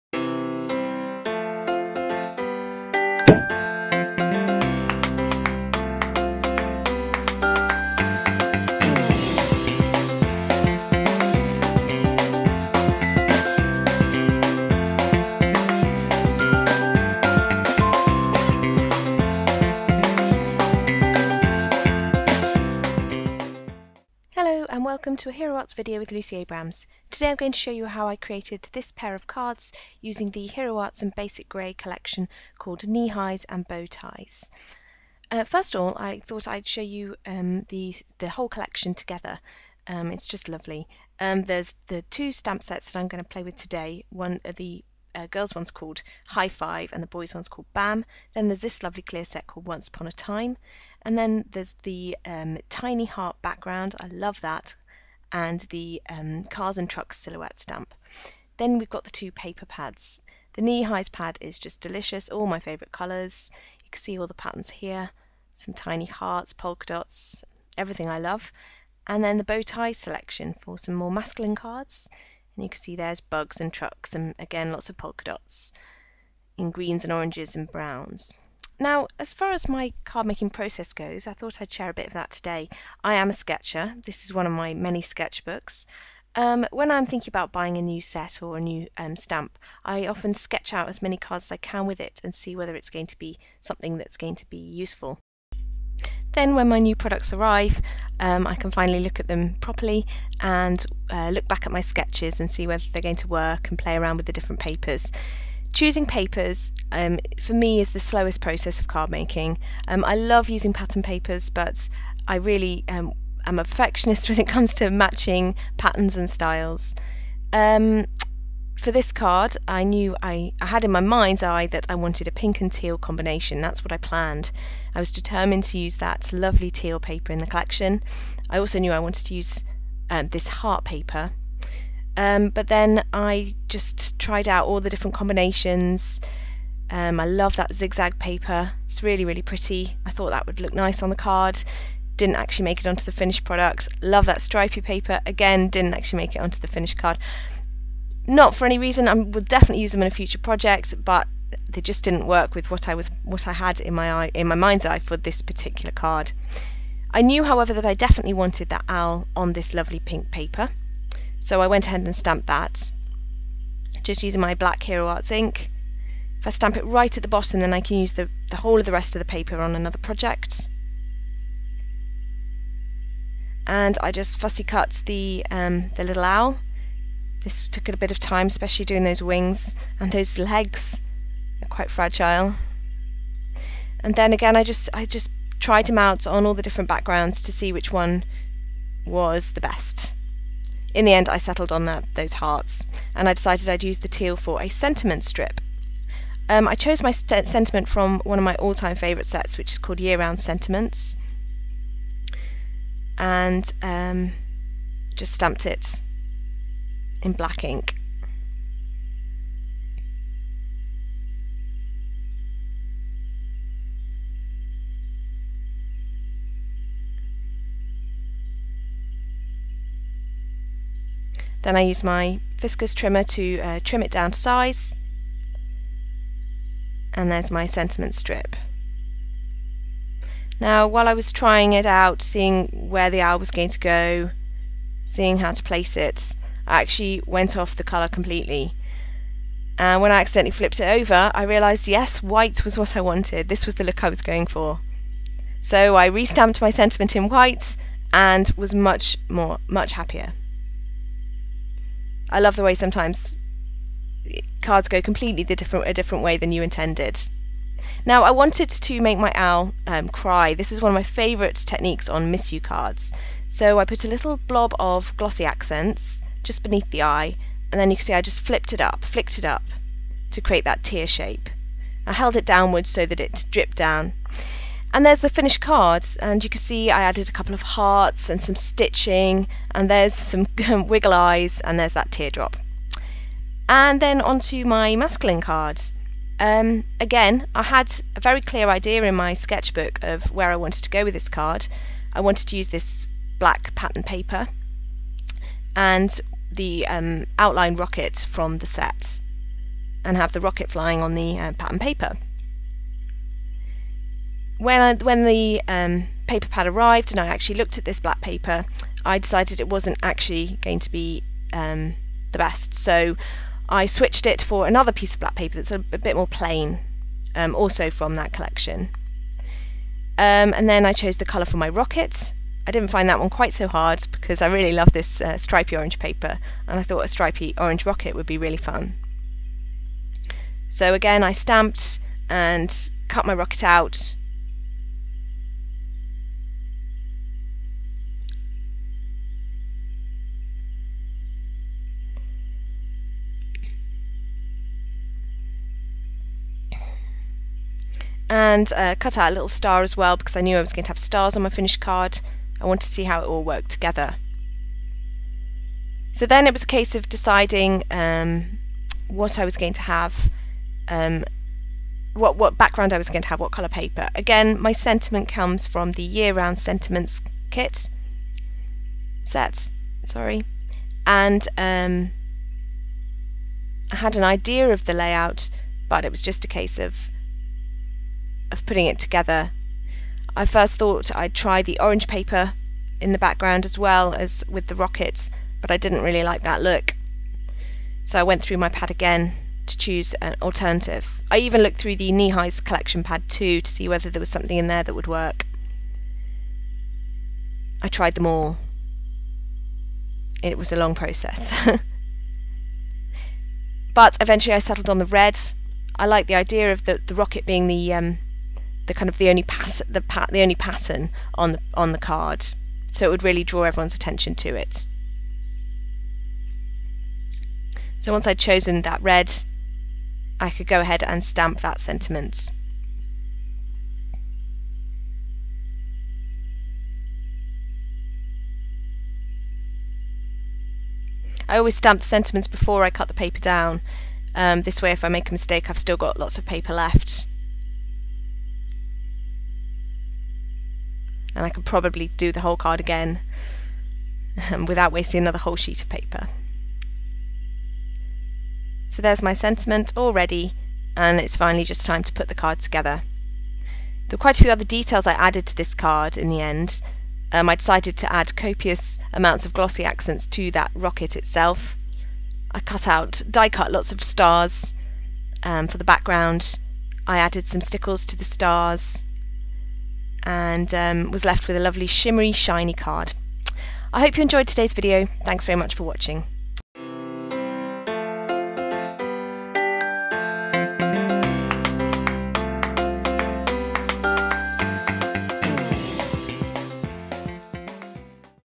Girl and Boy Cards with Kneehighs and Bowties
Lucy Abrams explains her design process and creates a pair of cards using the Kneehighs and Bowties stamps and papers. for more information.